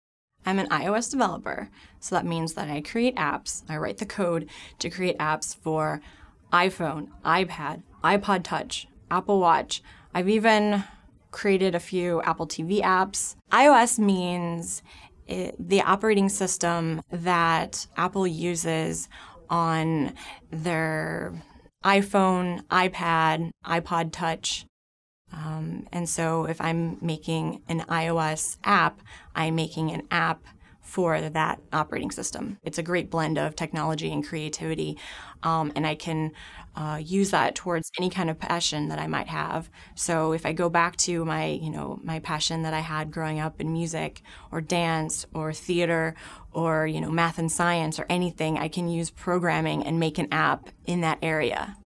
What I Do | IOS Developer Nichole Radman | Career Girls Role Model
Nichole Radman, an iOS developer, who creates apps for many different Apple devices. She finds her work very rewarding because she enjoys creating something that makes people’s lives easier. She also enjoys teaching young girls to code and create apps.